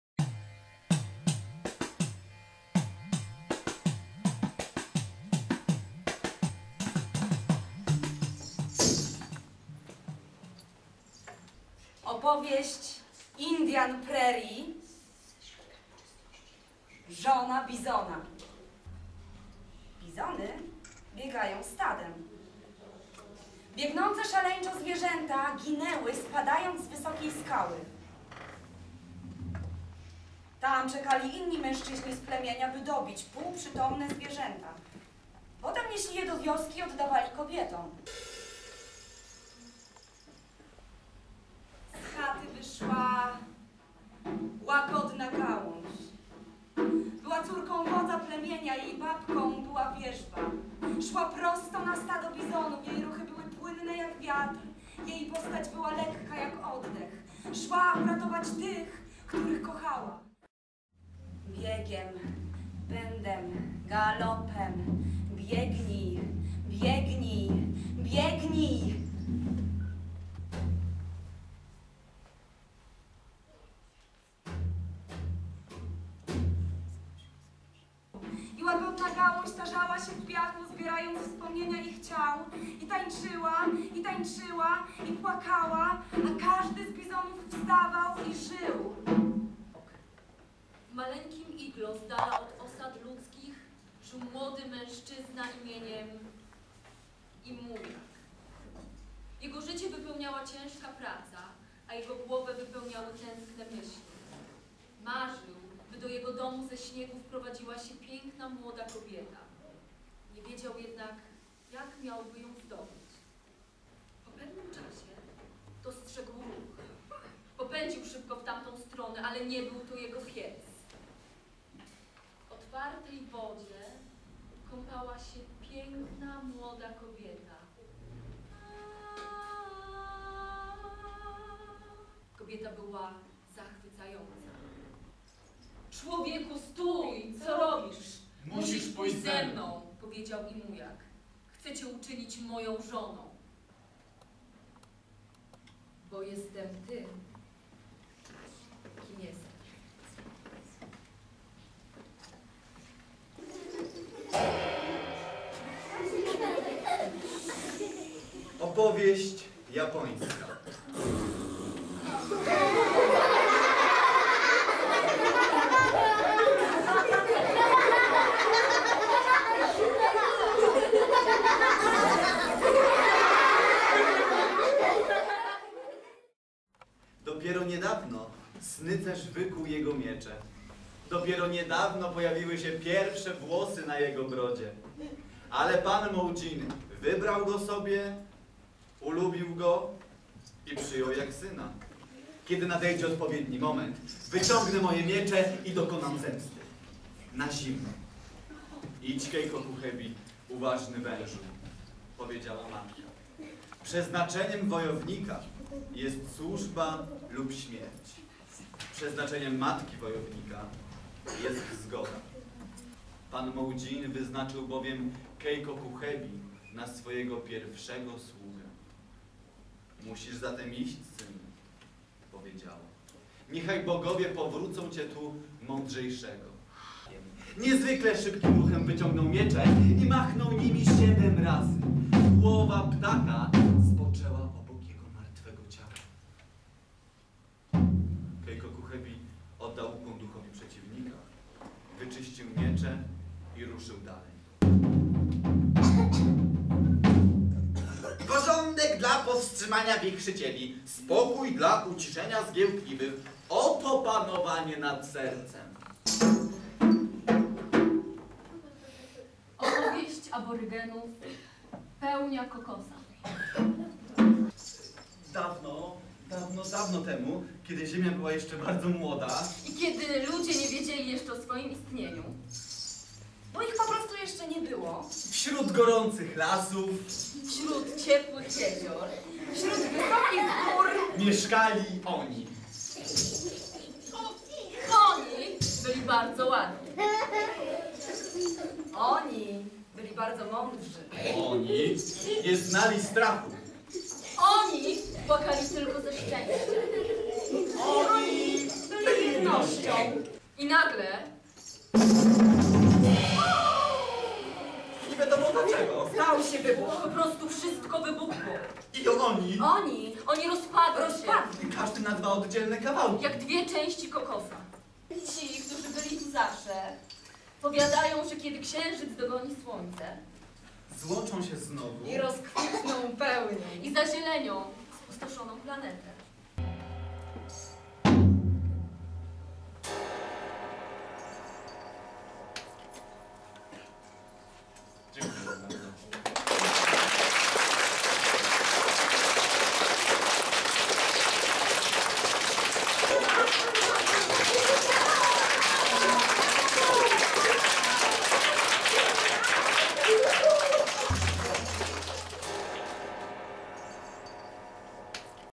Miejska Biblioteka Publiczna w Bolesławcu Teatr Prze(WRO)tny Opowieści z czterech stron świata
Przedstawienie skierowane do najmłodszych widzów składa się z czterech różnych opowieści pochodzących od czterech różnych narodów. Spektakl w nowoczesnej  formie teatru literackiego pokazuje wzajemne zależności między Ziemią a ludźmi ją zamieszkującymi.